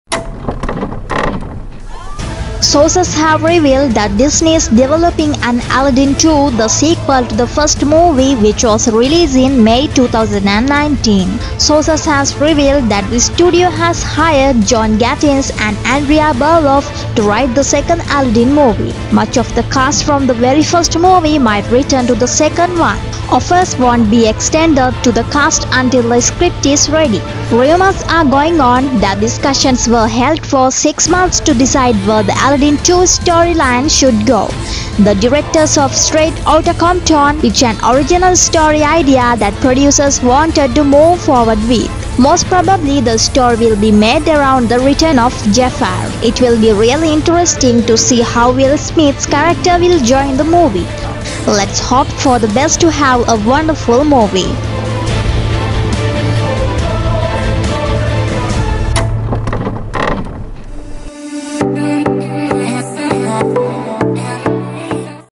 ALADDIN 2 - Disney Is Developing Aladdin 2 - Upcoming Disney Movie
Sources have revealed that Disney is developing an Aladdin 2,
the sequel to the first movie, which was released in May 2019.

Sources has revealed that the studio has hired John Gatins and Andrea Berloff to write the second Aladdin movie.

Much of the cast from the very first movie might return to the second one.

Offers won’t be extended to the cast until a script is ready.

Rumours are going on, that discussions were held for 6 months to decide where the Aladdin 2 story-line should go.

The directors of Straight Outta Compton, pitched an original story idea that producers wanted to move forward with.

Most probably the story will be made around the return of Jafar.

It will be really interesting to see how Will Smith's character will join the movie.

Let's hope for the best to have a wonderful movie...

Credits :
Walt Disney Studios